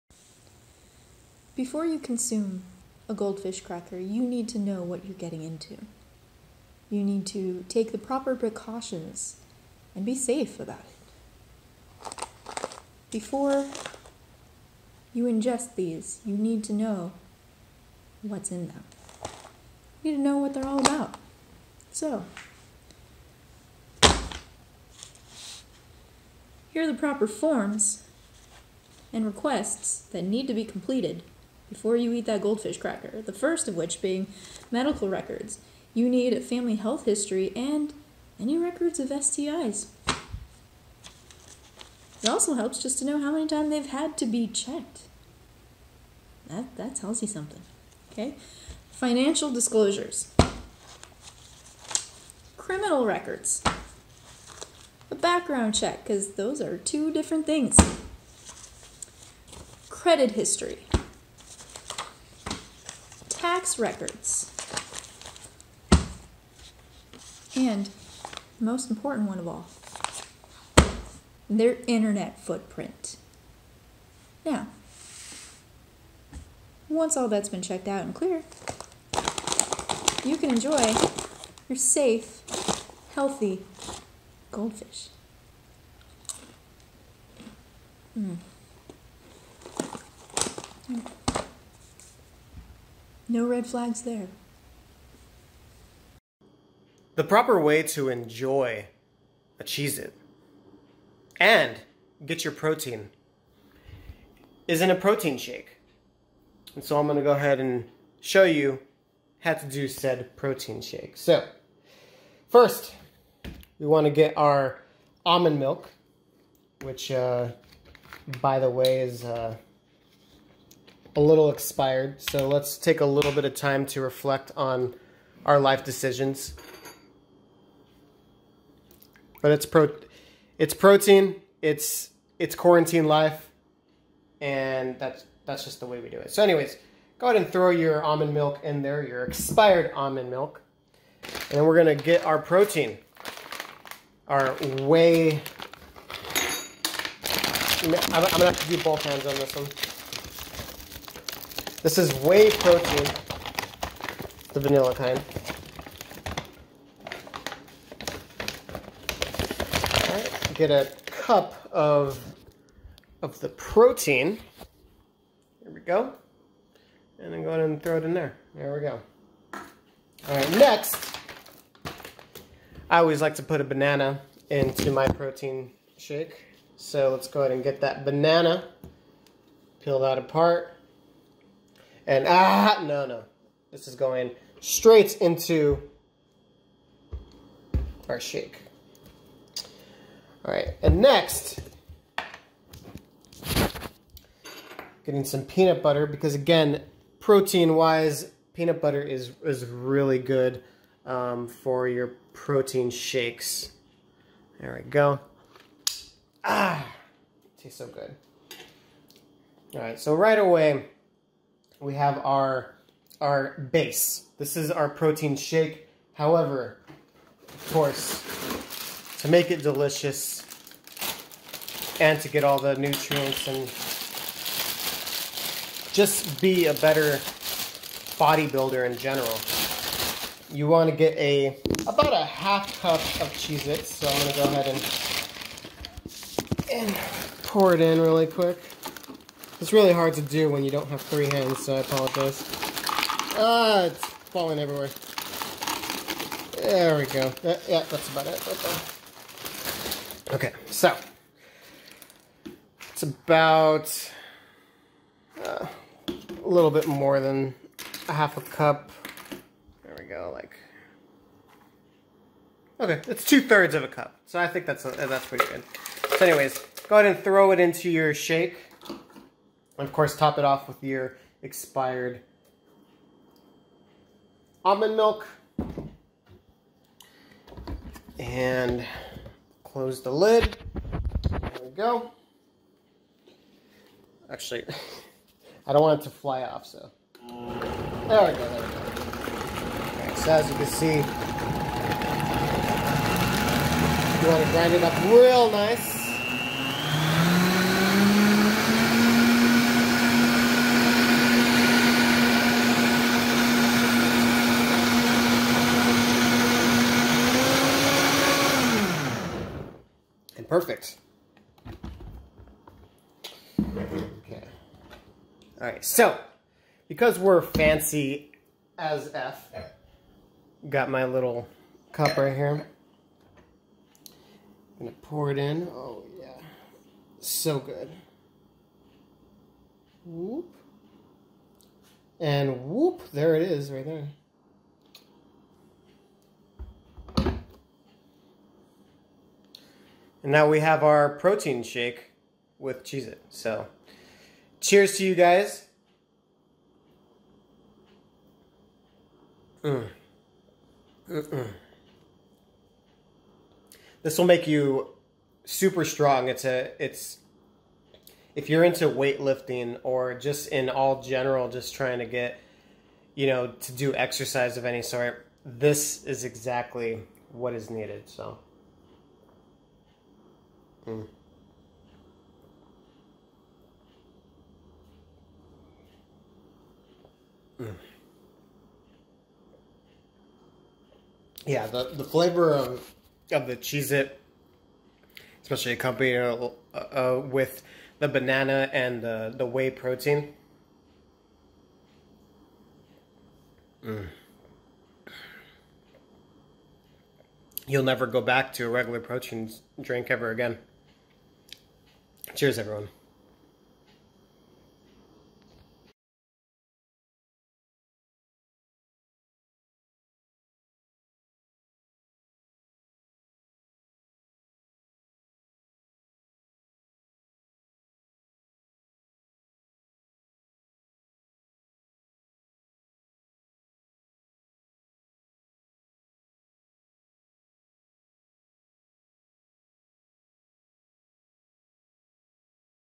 Proper Way to Eat Goldfish and Cheez-Its Saga: Part V
The Quarantine has officially drove us insane... Cassie sent a text video message to me of The Proper Way to Eat Goldfish.  So naturally, I responded back with a Proper Way to Eat a Cheez-It (as I didn't have Goldfish at the house).  Cassie sends me another response and so on and so on.  Finally, I ask... can I use these videos and post them on YouTube?  lol.

So, blame the Quarantine!  You have been warned!  Enjoy Part V of the Saga of How to Eat Goldfish and Cheez-Its.